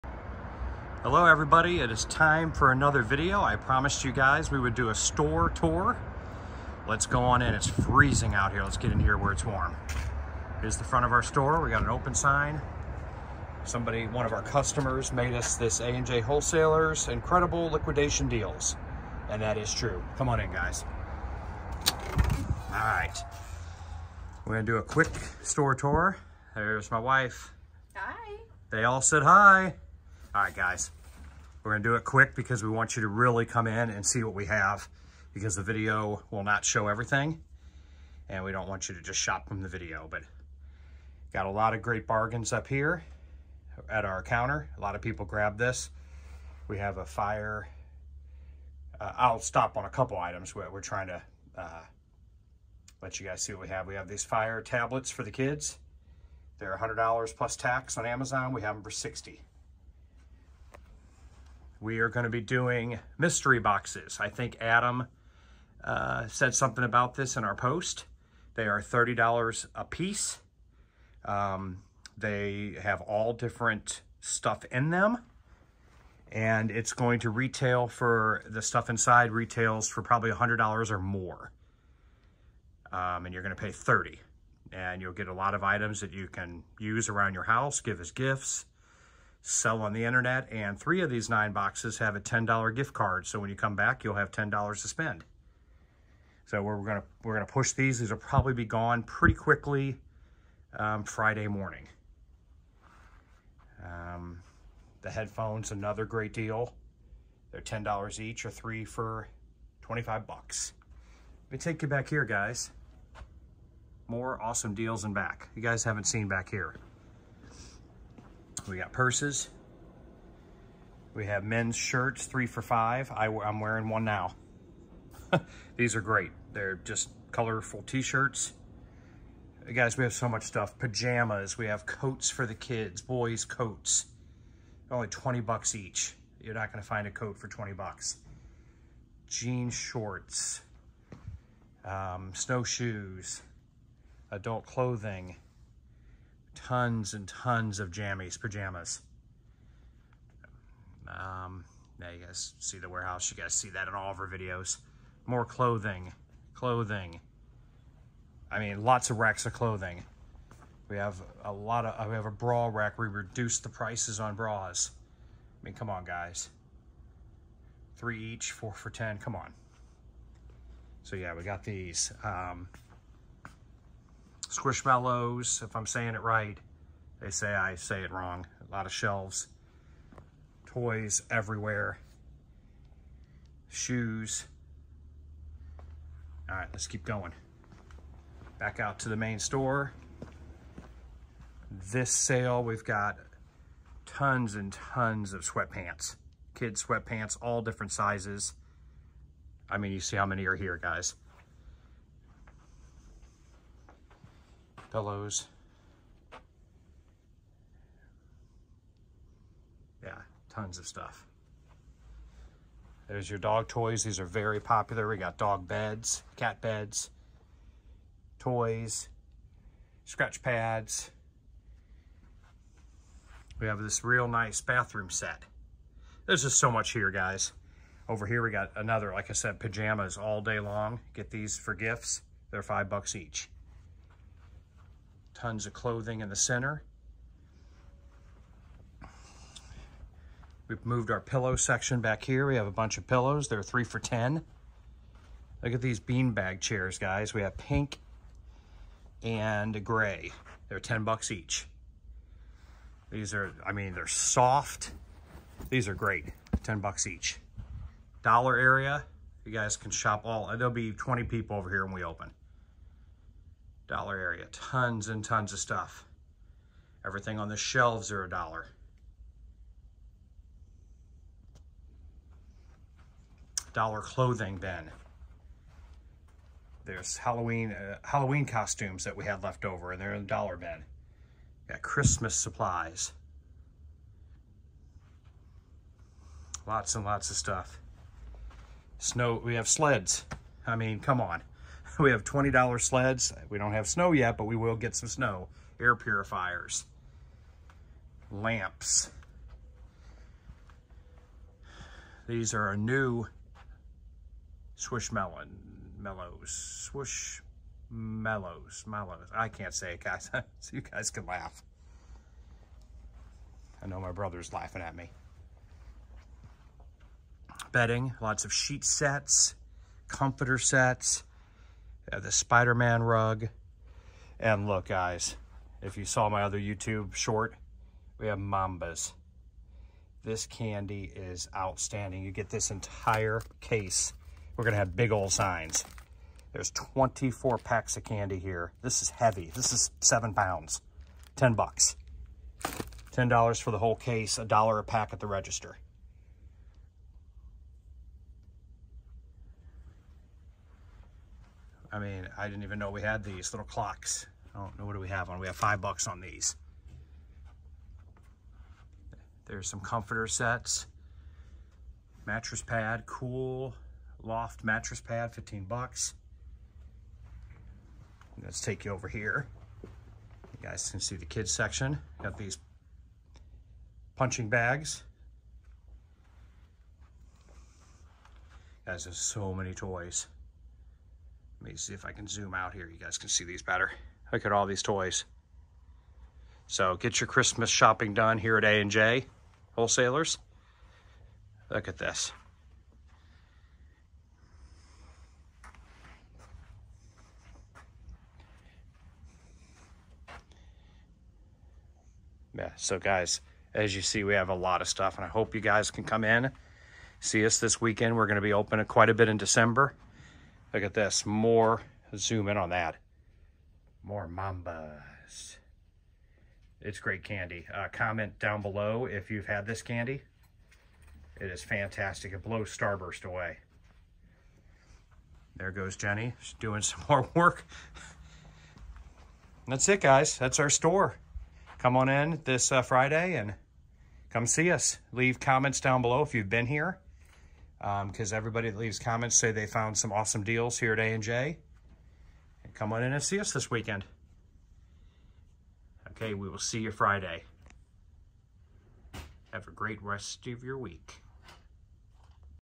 Retail store tour November 2022
Guys, you asked us for a video of the store so here it is this is the items that we will have in our store for the sale this Friday.￼